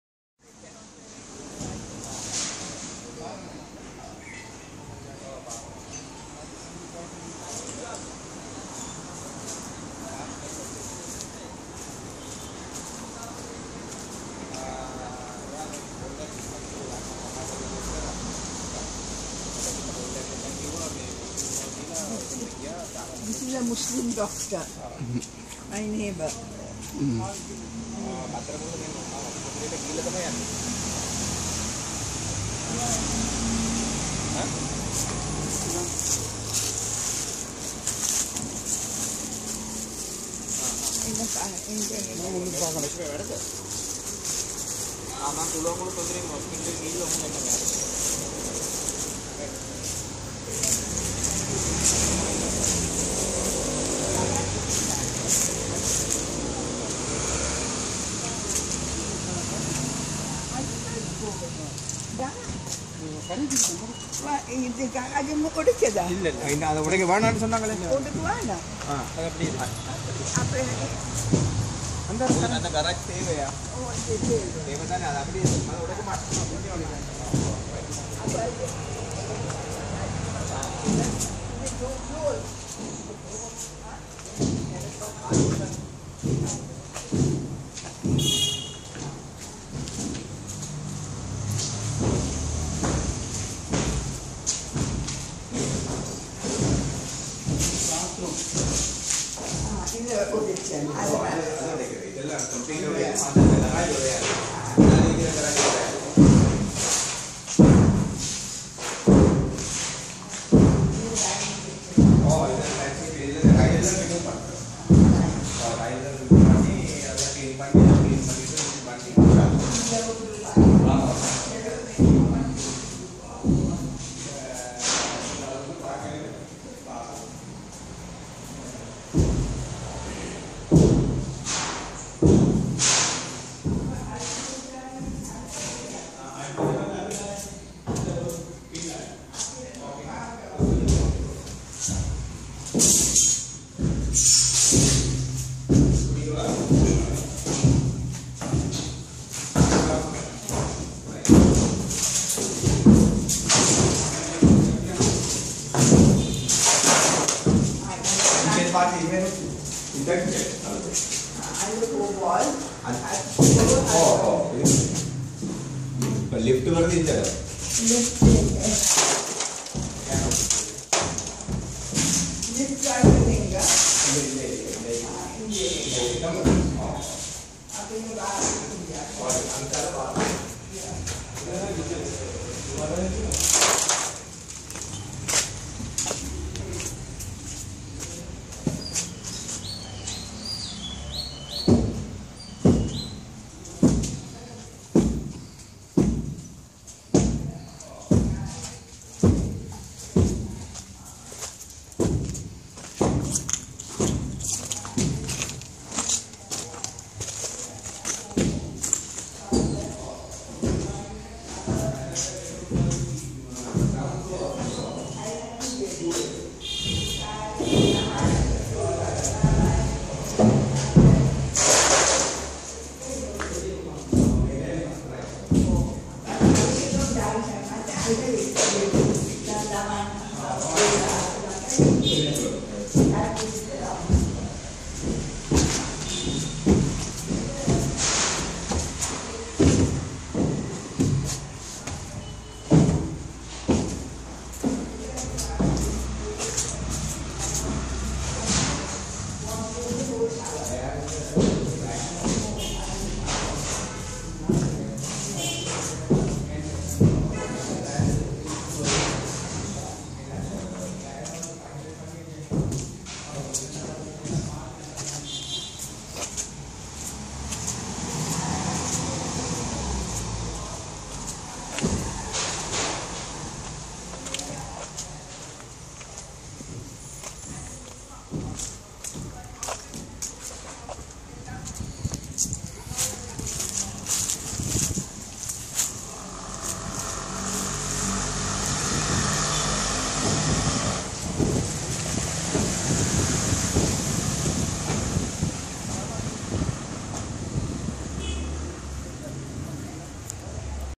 Building work in Sri Lanka
This is how building work is done in Sri Lanka. The new owner wants great changes to the building hence the work.